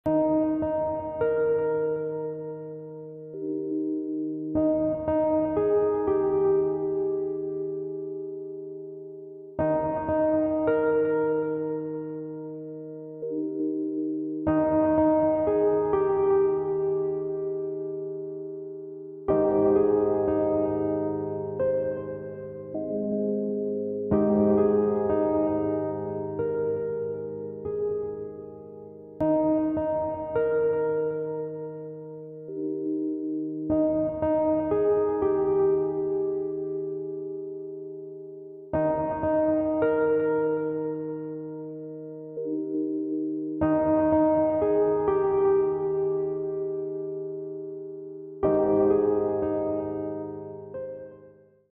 Sulphur butterflyes. Borboletas amarelas.
At the beginning of the rainy season (October/November) lots of butterflyes can be seen in the Pantanal.
No início das chuvas ( Outubro/Novembro) muitas borboletas podem ser vistas no Pantanal.